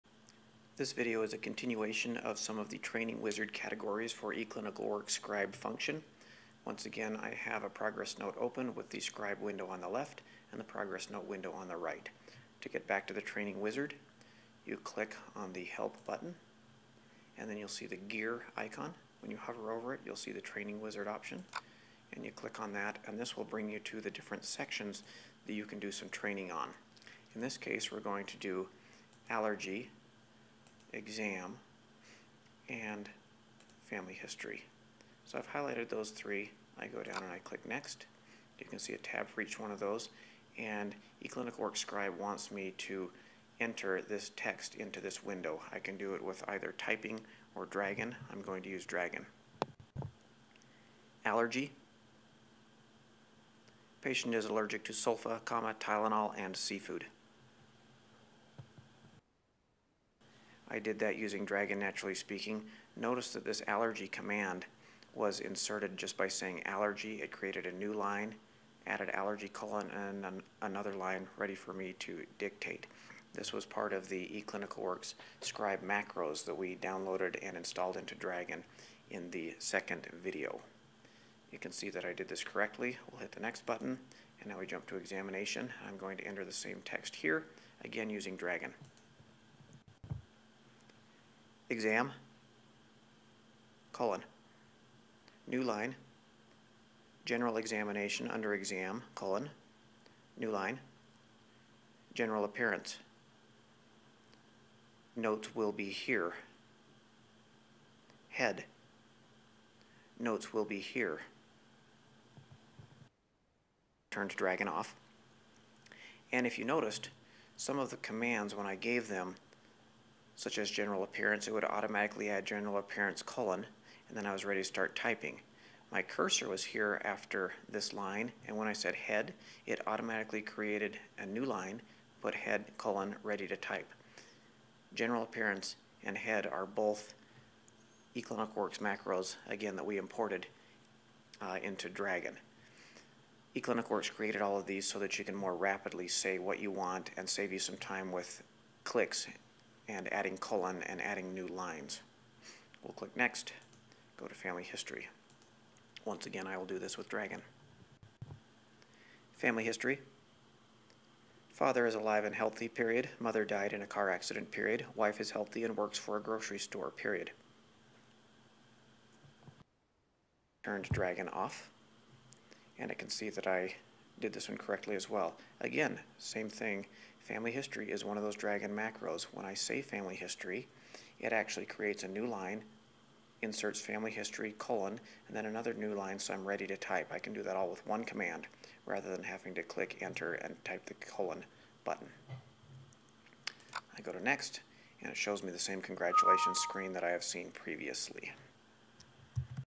eCW Scribe 5 Training Wizard Part 2
eClinicalWorks Scribe has a training wizard to help the user learn how to use some of the commands. This video shows a few areas of the training wizard.